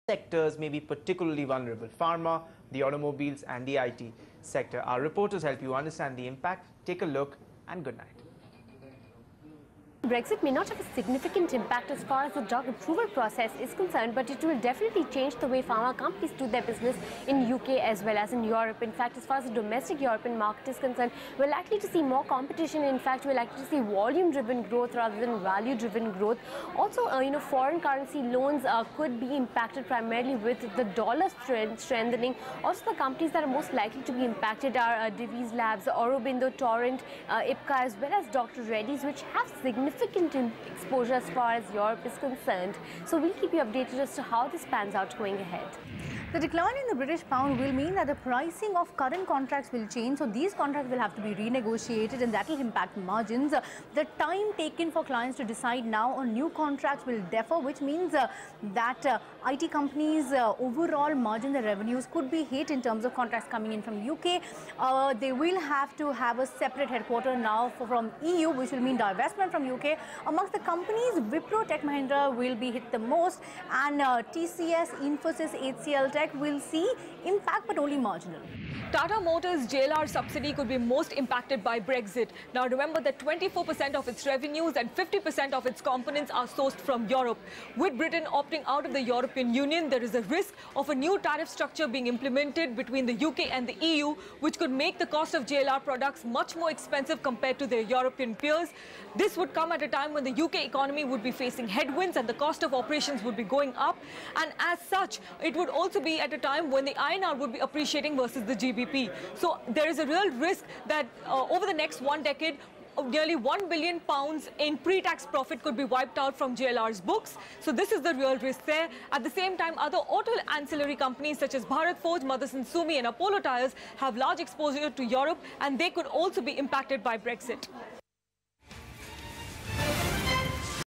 UKLeavesEU: Britain Walks Out, So Does Cameron. Britain Says Bye-Bye Brussels
So how will Britain’s exit from the EU impact Indian corporates. 3 sectors may be hit the most - Pharma, Auto & IT. Our reporters help you understand the impact. Take a look.